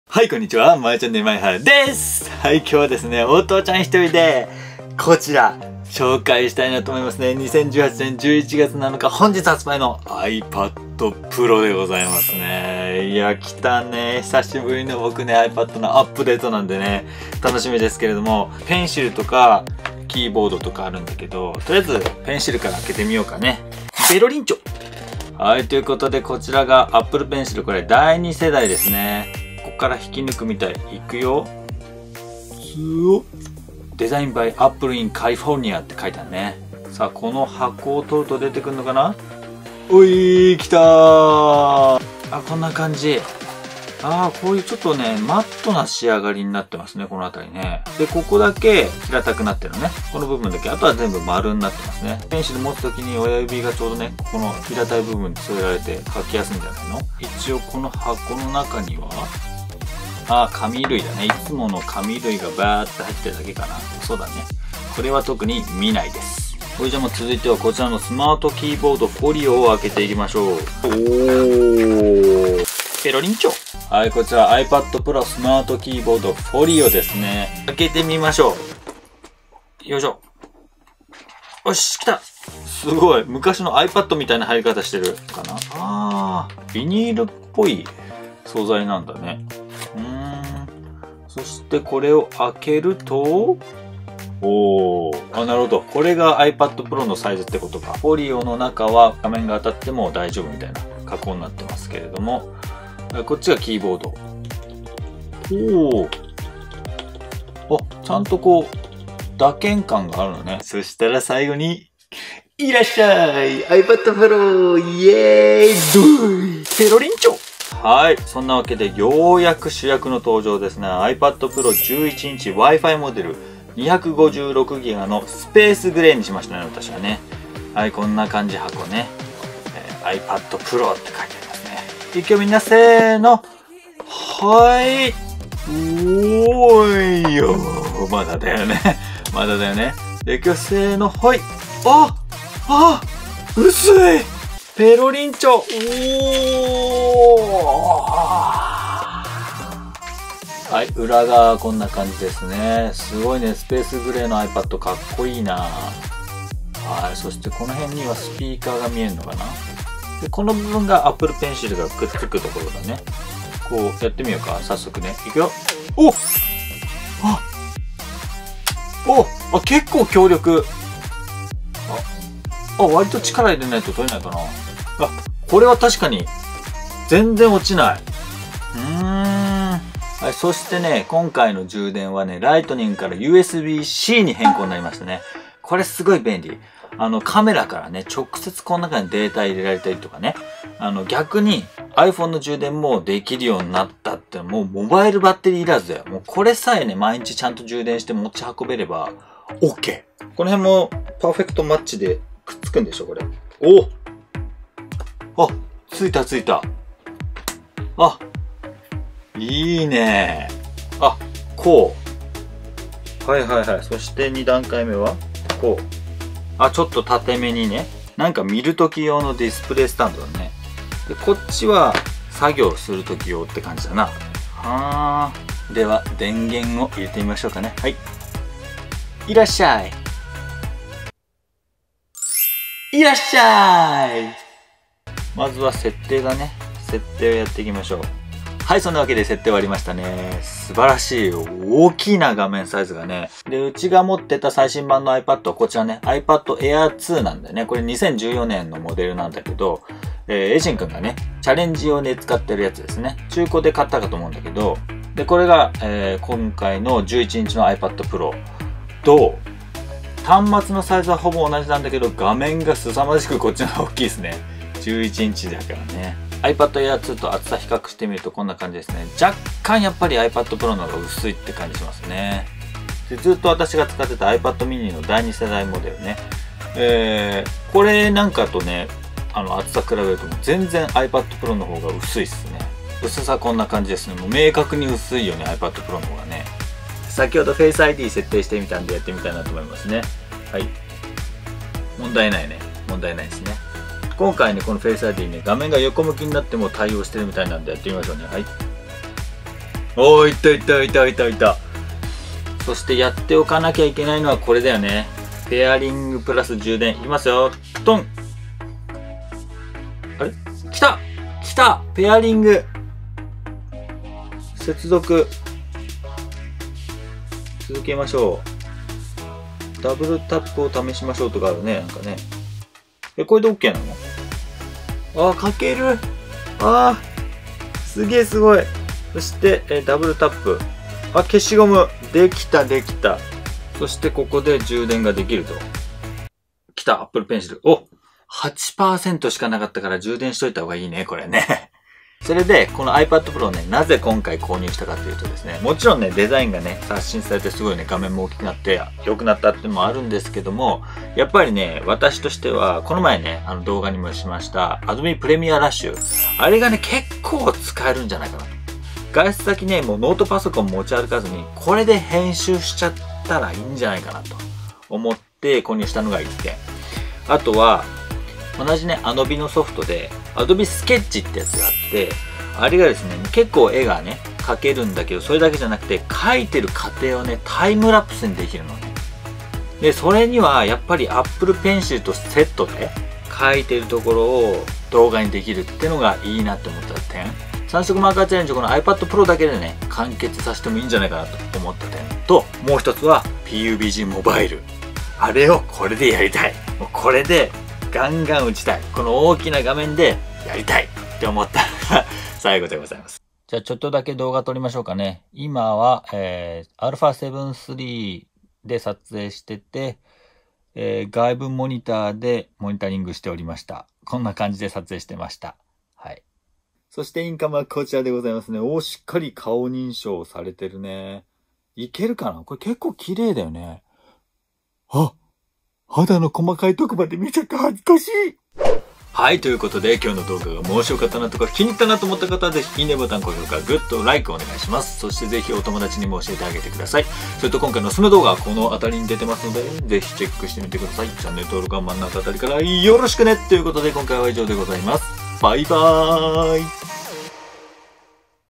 いらっしゃ〜い♪ iPad Pro 2018 Apple Pencilがカチッとくっつく! | まえちゃんねる
2018年11月7日本日発売の「iPad Pro」11インチ 256GB スペースグレー モデルを購入しました！
今回は開封して色々と設定してみました！とてもパワフルなので今後活躍してくれるはずです♪

▽まえちゃんねる Tシャツ 販売中♪